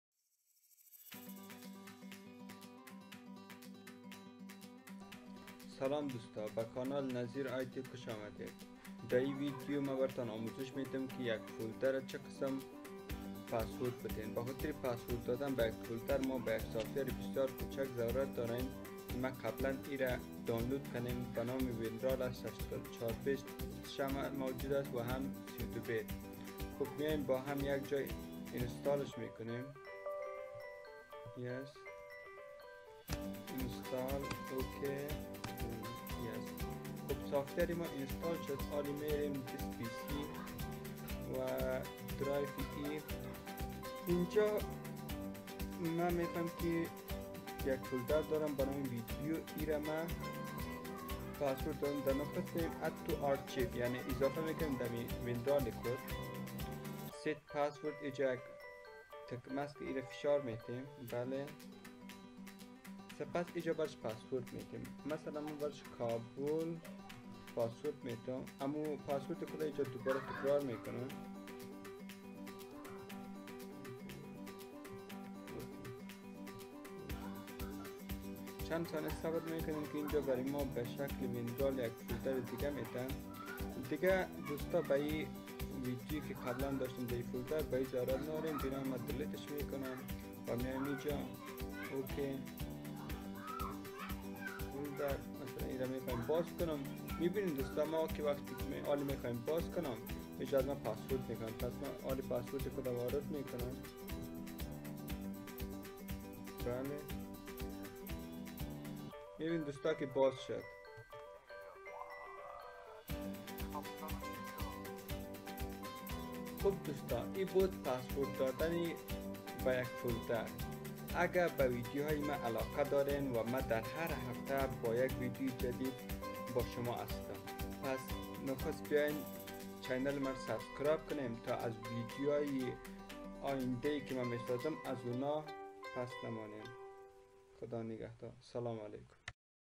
چطور به یک فولدر شخصی خود پسورد بدهیم
این ویدیو در مورد پاسورد دادن به فایل های شخصی است که به آسانی میتوانید به فایل های شخصی خود پاسورد بدهید